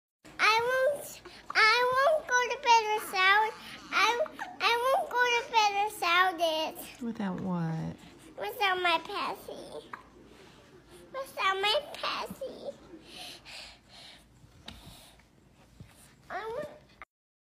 I won't go to bed without it 😫
Wynstan is a baby Red Handed Tamarin Monkey. He is the size of a finger monkey and will grow up to 1 lb when full grown. Izabella is a tiny 3 lb chihuahua puppy dog.